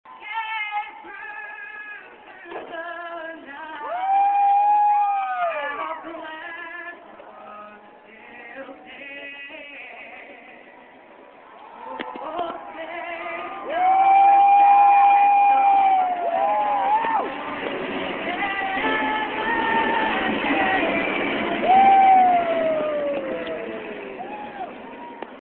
DOVER-BUSCH SERIES FLYOVER
machanical jet bird flys over race track.